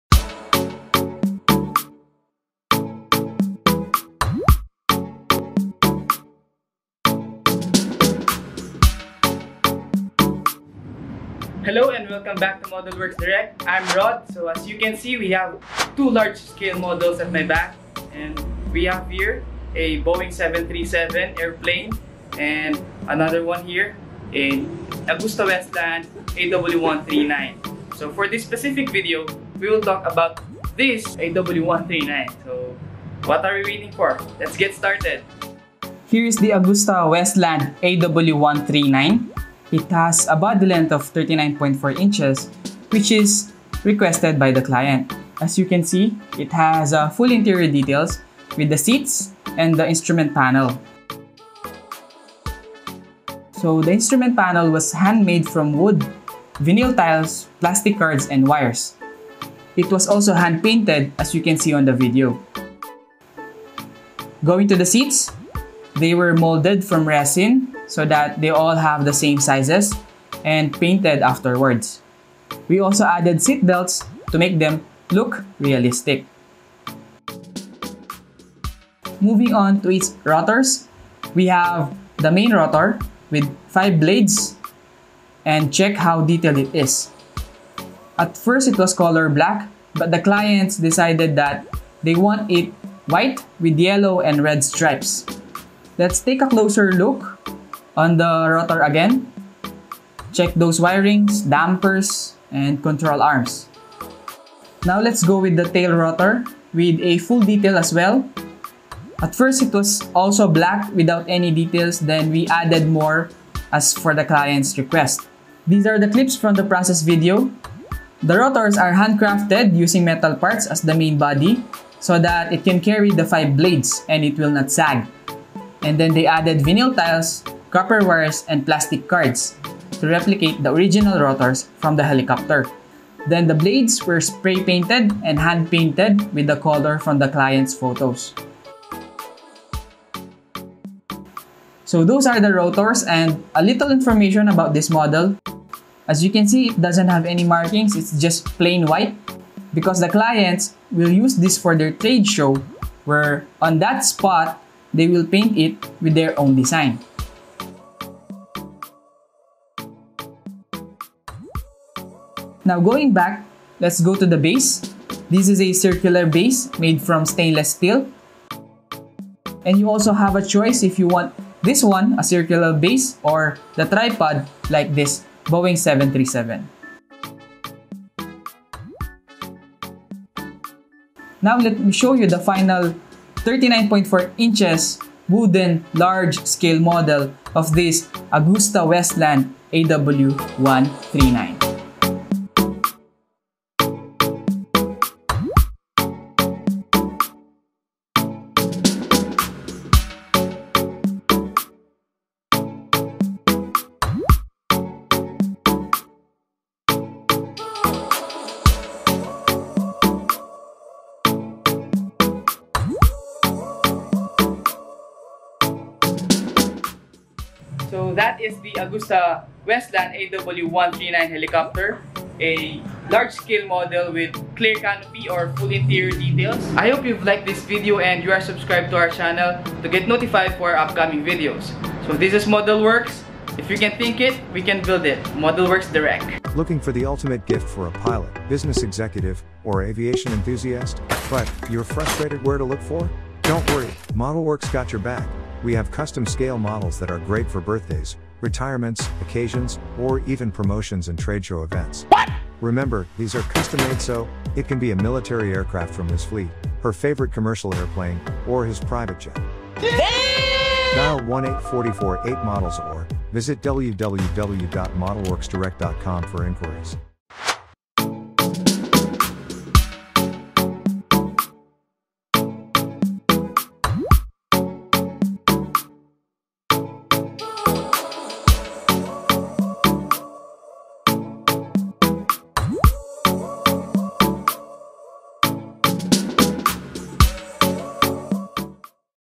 Giant Helicopter Model AW139. Blades Spinning. Super Detailed.
Giant Helicopter Model AW139. Blades Spinning. Super Detailed and Ready to Fly!    Large Custom Helicopter Model replicated from clients' photos.

👇 Toll-Free
➥ 1-844-8MODELS